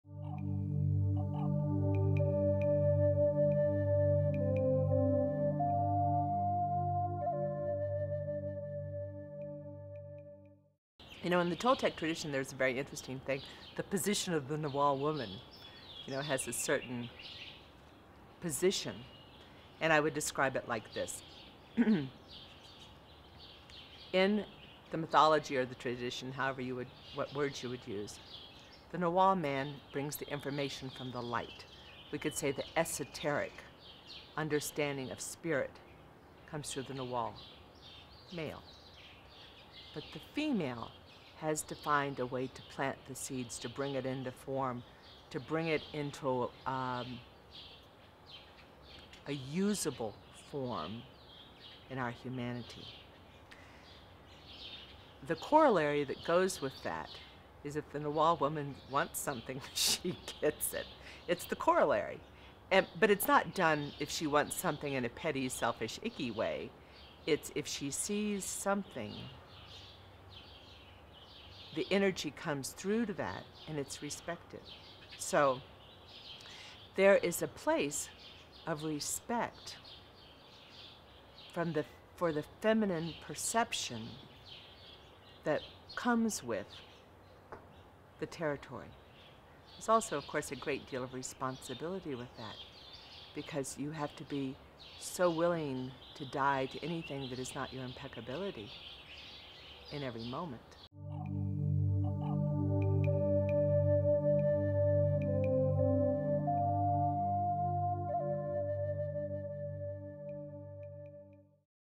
The Nagual Woman
Gini Gentry speaks about the Nagual from the Toltec tradition perspective.